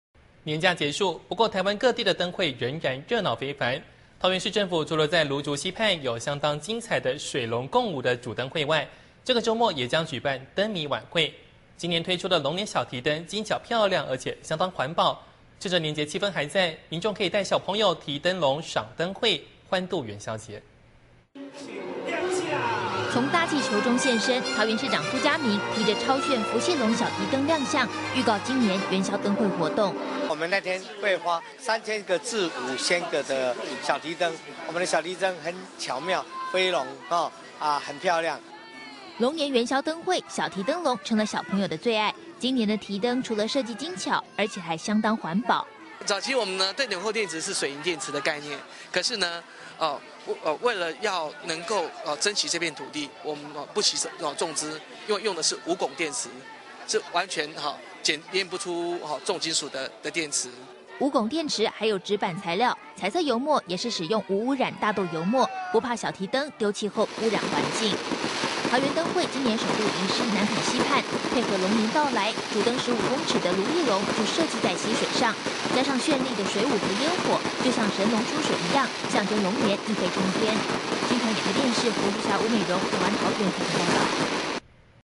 桃園環保小提燈 慶龍年元宵
【新唐人亞太台2012年2月3日訊】年假結束，不過台灣各地的燈會仍然熱鬧非凡，桃園市政府除了在蘆竹溪畔，有相當精彩的水龍共舞的主燈會外，這個週末也將舉辦燈謎晚會，今年推出的龍年小提燈精巧漂亮，而且相當環保，趁著年節氣氛還在，民眾可以帶小朋友提燈籠賞燈會，歡度元宵節。

從大氣球中現身，桃園市長蘇家明提著超炫「福氣龍」小提燈亮相，預告今年元宵燈會活動。

桃園市長 蘇家明：「我們那天會發三千個至五千個小提燈， 我們的小提燈很巧妙，飛龍很漂亮。」

龍年元宵燈會小提燈籠成了小朋友的最愛，今年的提燈除了設計精巧，而且還相當環保。

提燈設計師：「早期對鈕釦電池是水銀電池的概念，可是為了要能夠珍惜這片土地，我們不惜重資用的是無汞電池，是完全檢驗不出重金屬的電池。」

無汞電池還有紙板材料，彩色油墨也是使用無污染大豆油墨，不怕小提燈丟棄後污染環境。

桃園燈會今年首度移師南崁溪畔，配合龍年到來，主燈15公尺的「蘆意龍」就設計在溪水上，加上絢麗的水舞跟煙火，就像神龍出水一樣，象徵龍年一飛沖天。

新唐人亞太電視 胡淑霞 吳美蓉台灣桃園報導

其他推薦:
《神韻》《SHEN YUN》